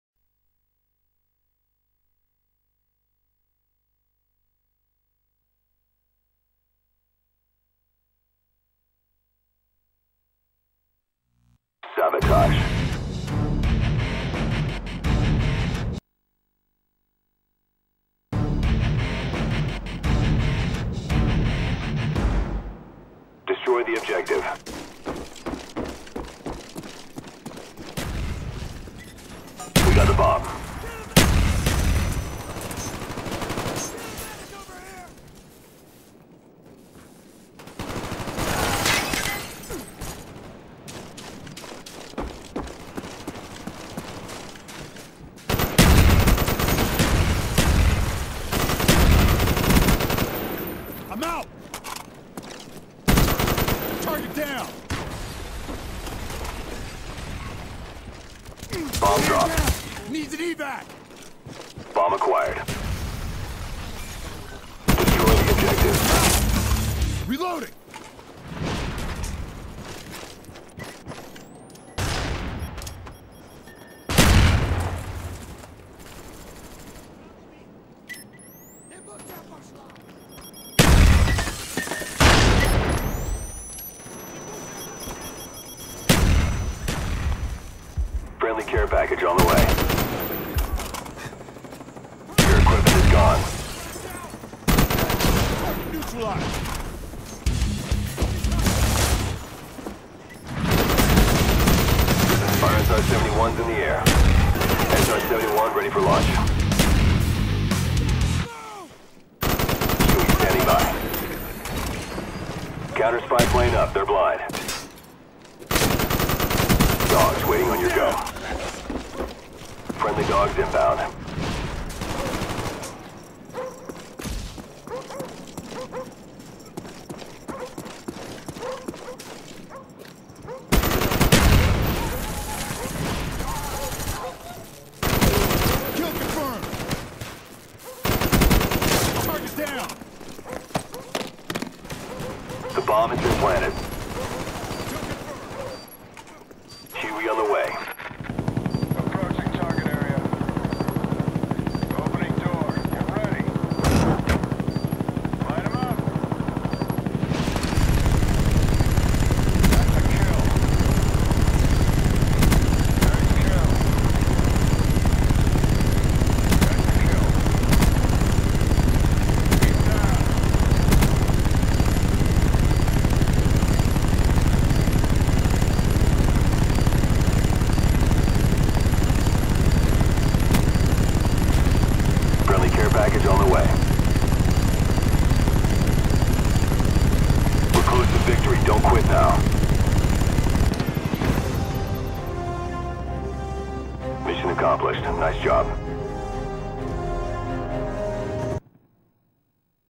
Quick 27-1 Sabo game
First Upload in a while, Good game with the Commando. Sorry for quality it will improve.